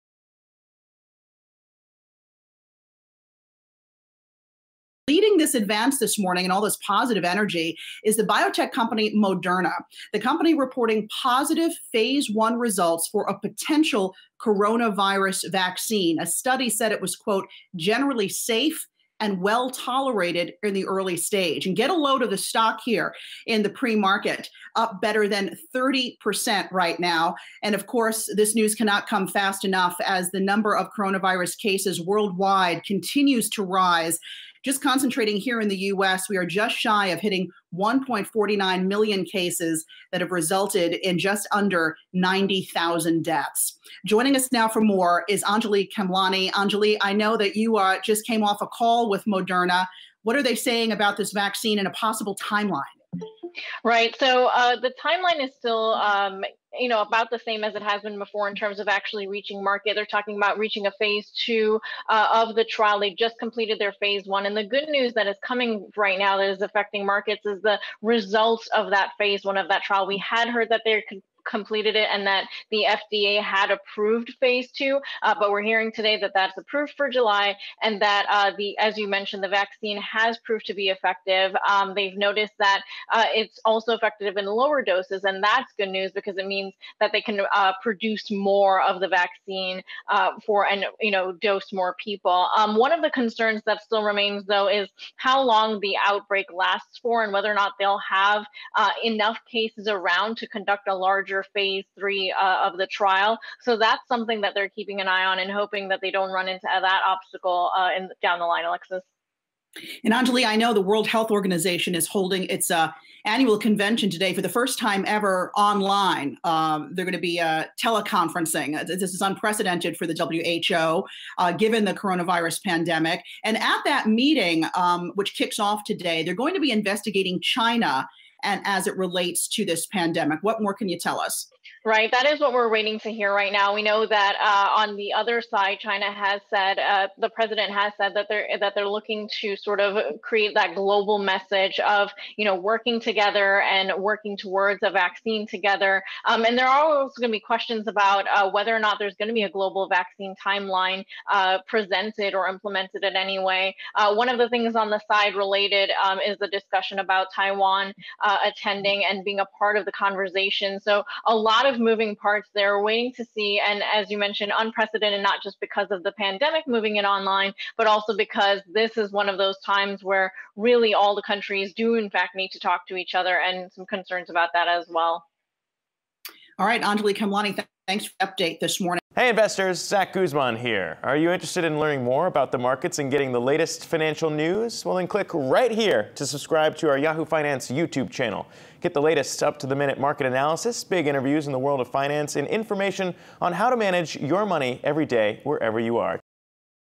Moderna reports positive early vaccine trial results
Senior Reporter Anjalee Khemlani reports on the latest information on the coronavirus pandemic and the Moderna COVID-19 vaccine trials.